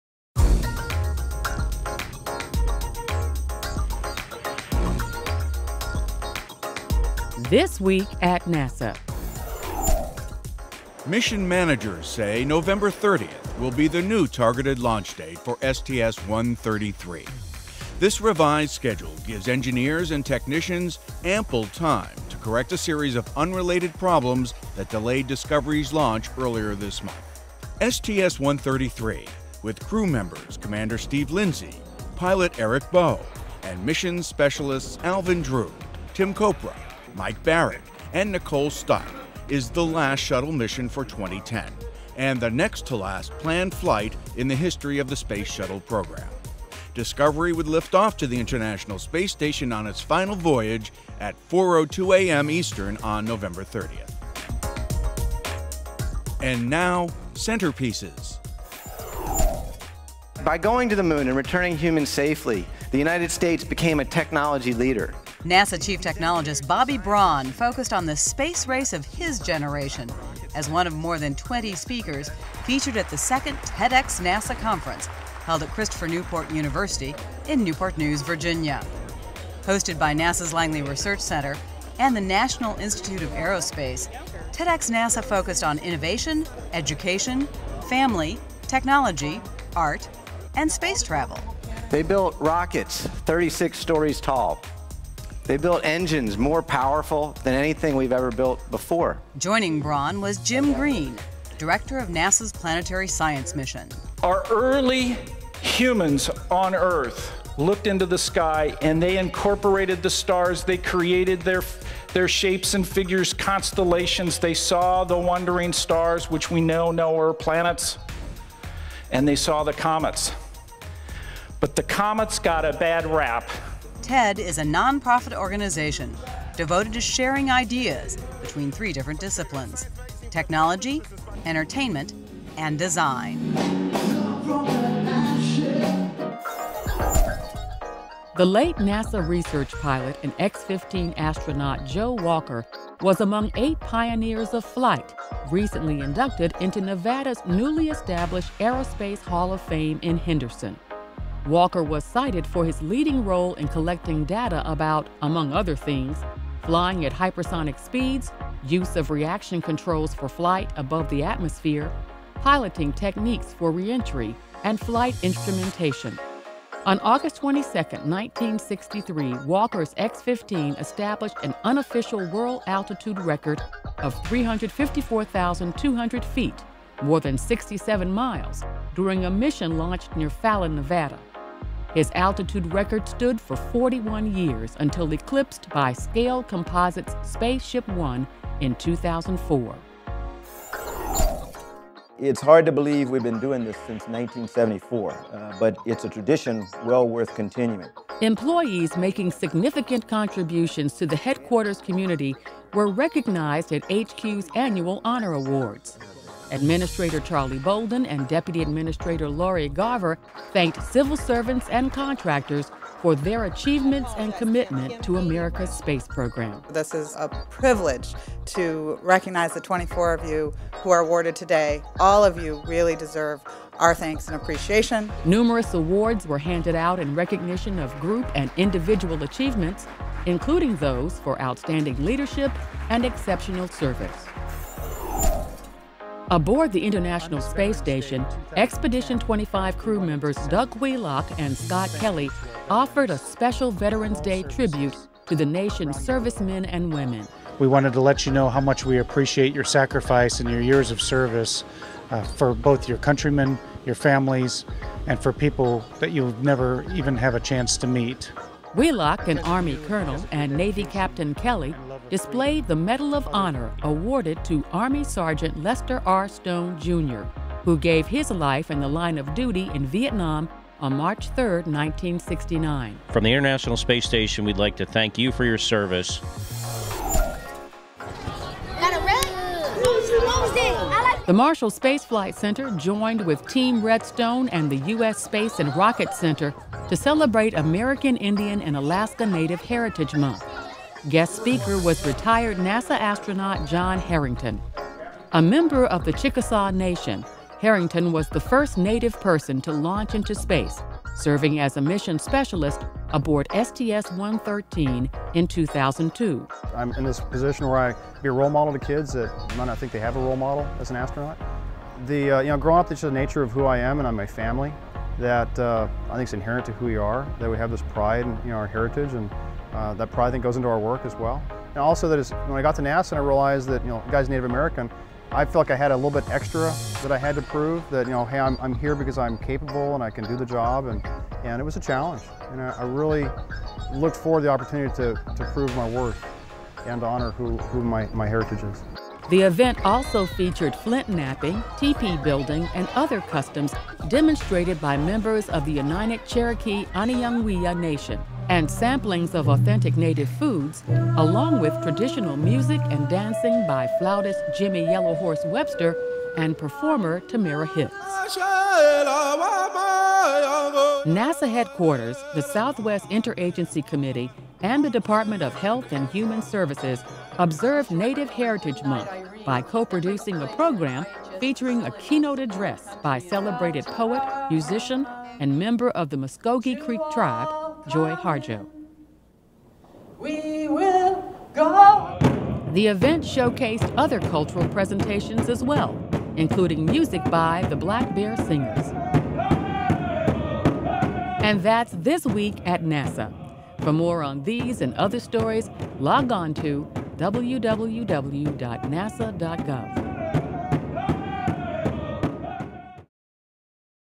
New Shuttle Launch Date Tops This Week
A new target launch date has been selected for STS-133, allowing ample time for repairs to space shuttle Discovery. Also, NASA's chief technologist and planetary science director were among more than 20 speakers featured at the second TEDxNASA conference in Newport News, Virginia. Hosted by the Langley Research Center, TEDx focused on education, innovation, family, technology, art and space travel. Plus, X-15 astronaut Joe Wagner joins Aerospace Hall of Fame; HQ Honor Awards; and Herrington helps celebrate American Indian & Alaska Native Heritage Month.